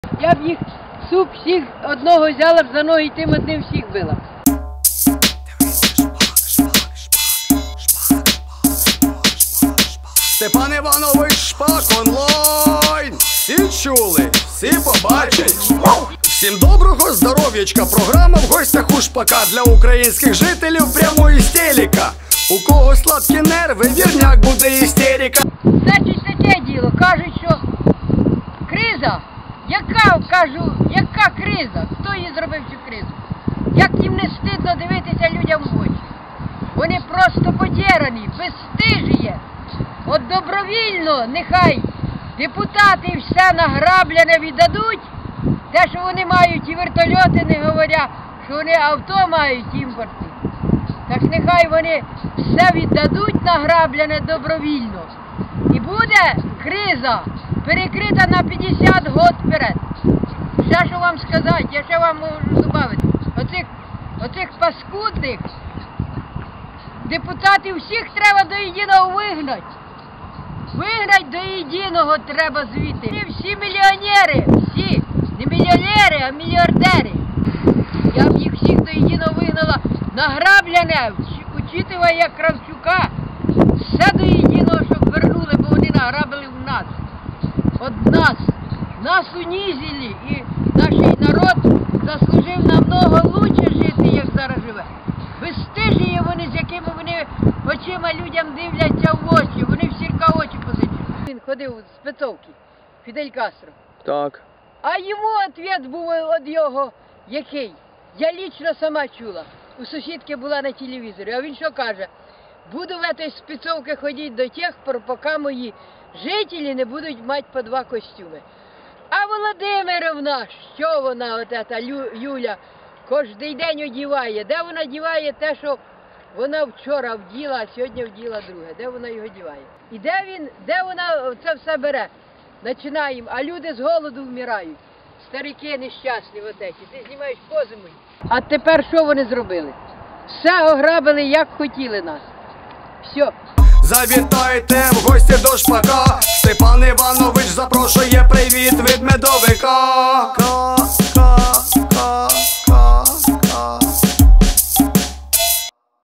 СИШ - кризис, депутаты, фидэль кастро, юля
СИШ - в гостях у Шпака : кризис, депутаты, фидэль кастро, юля